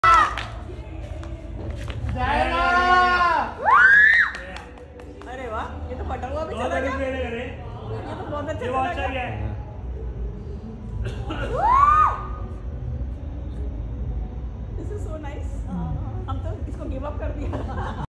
Mesmerising Chinese Sky Lantern lamp कण्डीला Flying Light Diwali
Chinese sky lantern mesmerising flight in sky
कण्डीला we use to fly in our childhood by tying it to kite